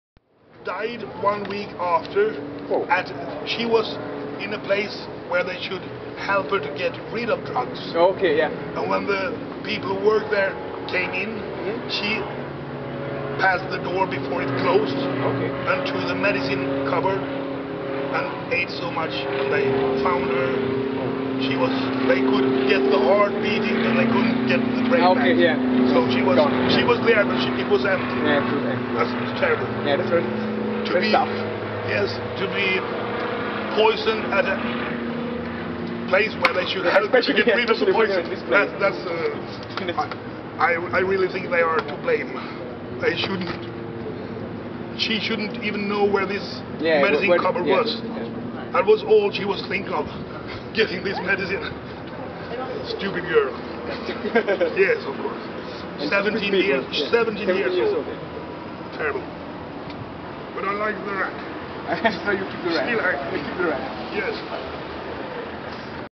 Song of Tomorrow - Sven Olof Molin @ Film festival di Locarno
Abbiamo incontrato Sven Olof Molin (dal film Song of Tomorrow) vicino alla zona accrediti che canticchiava alcune canzoni con la sua chitarra e racconta di alcuni personaggi... Ovviamente ci siamo fermati ad ascoltarlo...